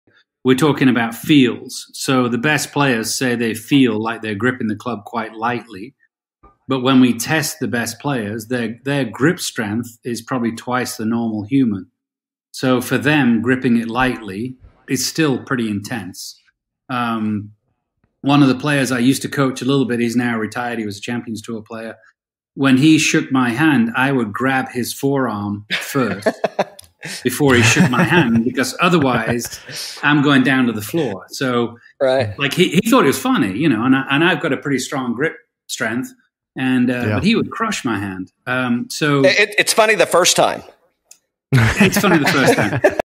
Jon Tattersall explains the golf grip and the misconception of gripping the club too lightly.
Most tour players have a grip strength twice that of a normal golfer. If you are following the advice of gripping the club lighter, you may be gripping it too light. Jon Tattersall explains gripping the club in detail on the Birdies & Bourbon podcast.